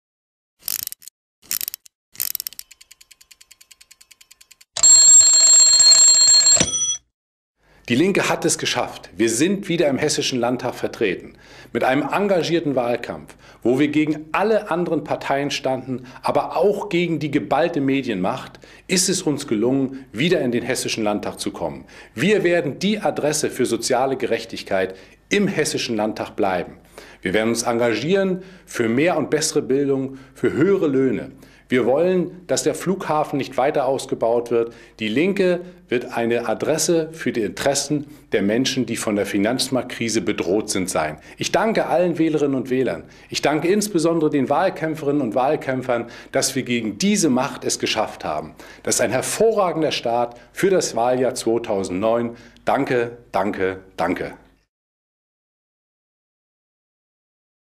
DIE LINKE: Dietmar Bartsch zum Ausgang der Wahl in Hessen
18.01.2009 - DIE LINKE ist wieder im hessischen Landtag vertreten. Mit einem engagierten Wahlkampf gegen alle anderen Parteien und gegen die geballte Medienmacht ist der Wiedereinzug gelungen. DIE LINKE wird die Adresse für soziale Gerechtigkeit im hessischen Landtag bleiben. Ich danke allen Wählerinnen und Wählern, allen Wahlkämpferinnen und Wahlkämpfern.